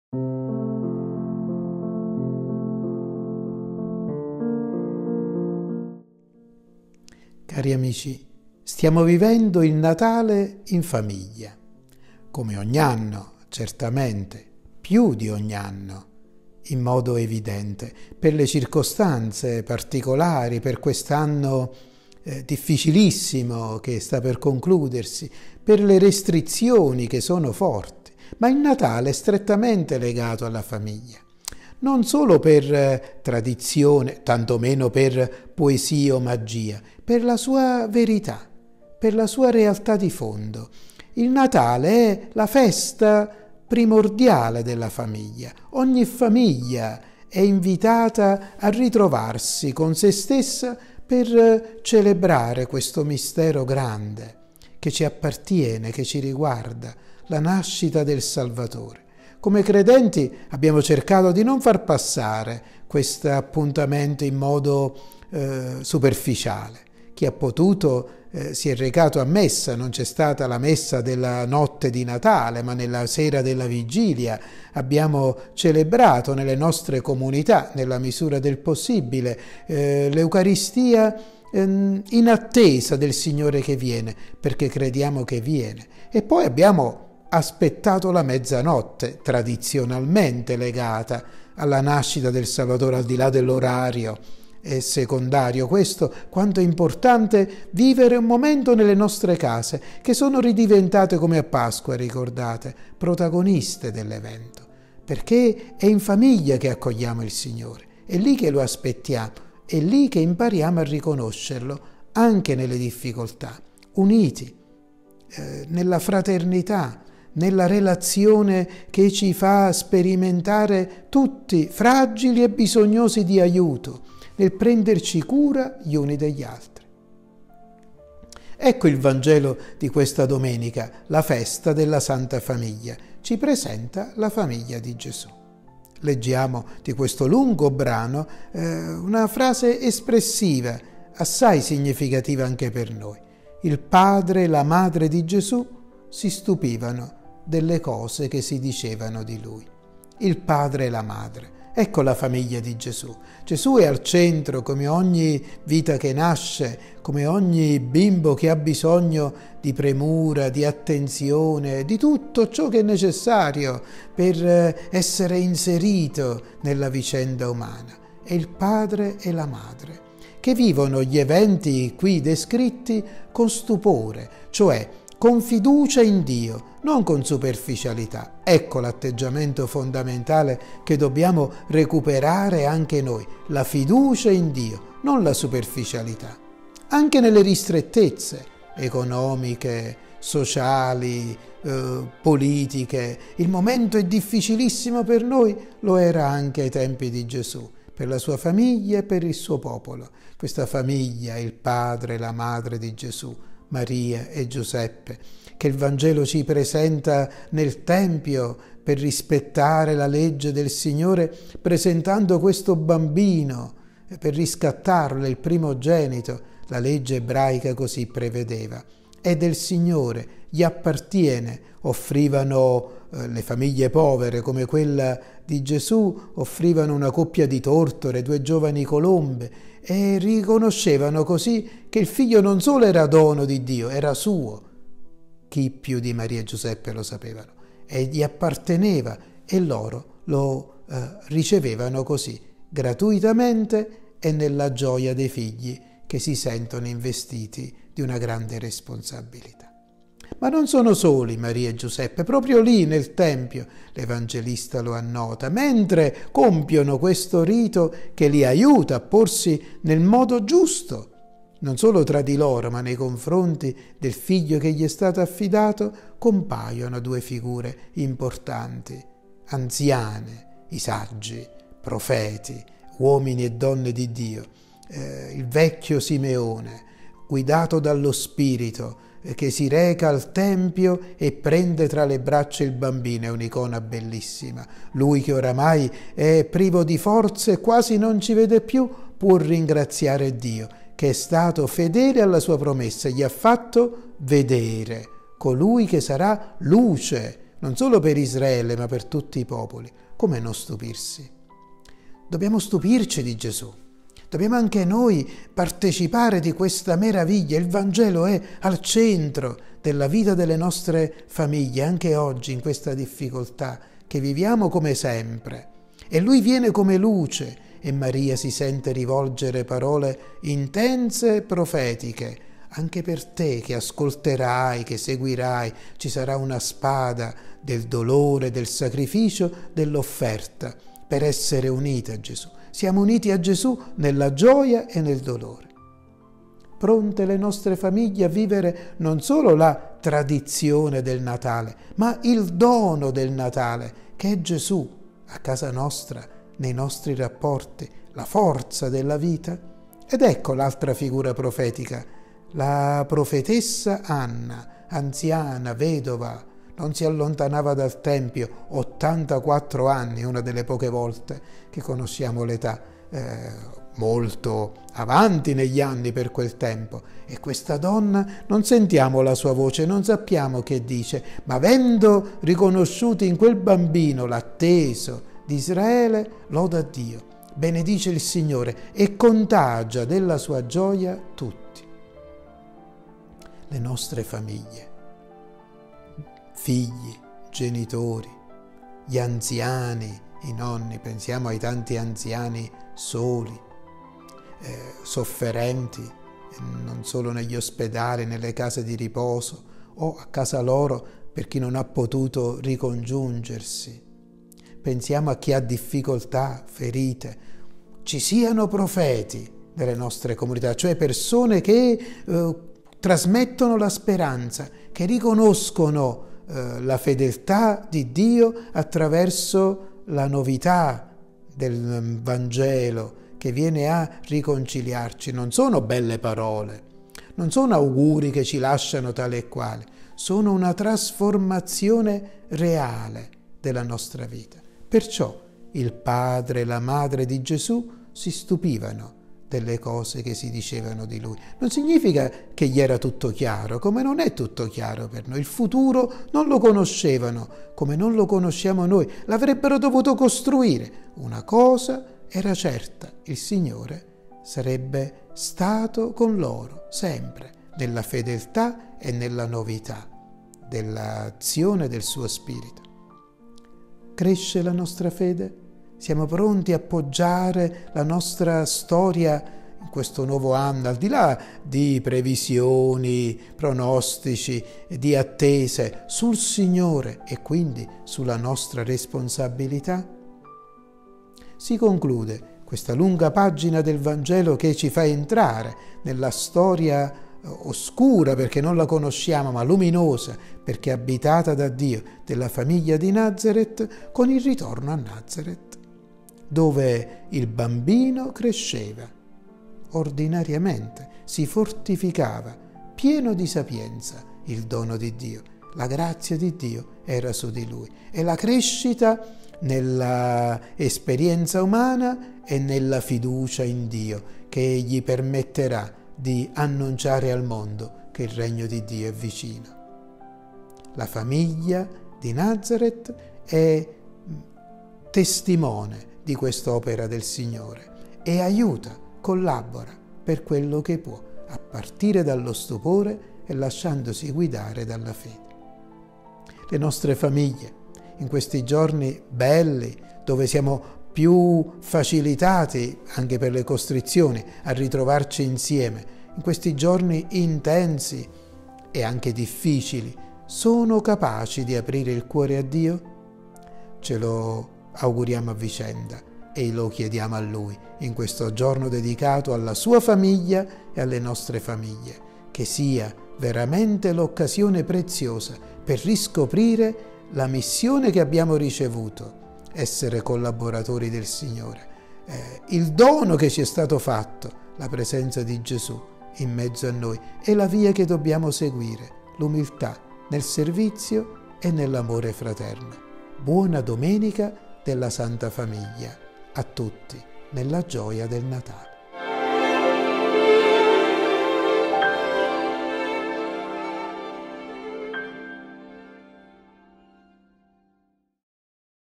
27 dicembre 2020
Appuntamento con la Parola Festa della Sacra Famiglia 27 dicembre 2020

"Il padre e la madre di Gesù si stupivano delle cose che si dicevano di lui."

Il nostro Vescovo Francesco, ci accompagna in questo brano del Vangelo.
Buona domenica!